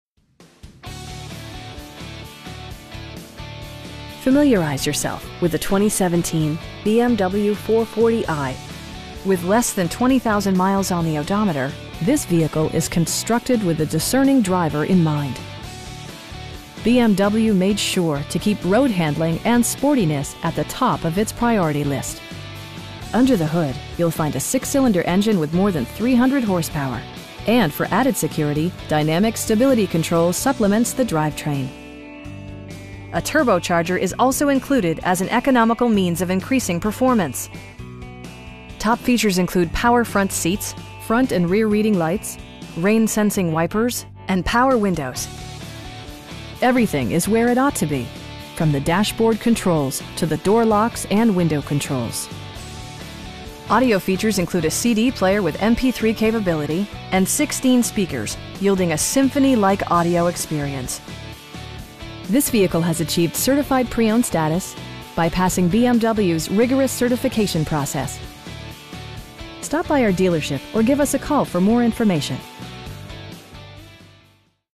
2017 BMW 4 Series 440i Convertible in Lakeland, FL 33809
Familiarize yourself with the 2017 BMW 440i With less than 20,000 miles on the odometer, this vehicle is constructed with the discerning driver in mind. BMW made sure to keep road-handling and sportiness at the top of it's priority list. Under the hood you'll find a 6 cylinder engine with more than 300 horsepower, and for added security, dynamic Stability Control supplements the drivetrain. A turbocharger is also included as an economical means of increasing performance. Top features include power front seats, front and rear reading lights, automatic dimming door mirrors, heated seats, high intensity discharge headlights, rain sensing wipers, blind spot sensor, and power windows. Everything is where it ought to be, from the dashboard controls to the door locks and window controls. Audio features include a CD player with MP3 capability, steering wheel mounted audio controls, A 20 gigabyte hard drive, and 16 speakers, providing excellent sound throughout the cabin. Passengers are protected by various safety and security features, including: knee airbags, front side impact airbags, traction control, anti-whiplash front head restraints, a security system, an emergency communication system, and 4 wheel disc brakes with ABS. Brake assist technology provides extra pressure when applying the brakes. This vehicle has achieved Certified Pre-Owned status, by passing BMW's rigorous certification process. We have the vehicle you've been searching for at a price you can afford. Stop by our dealership or give us a call for more information.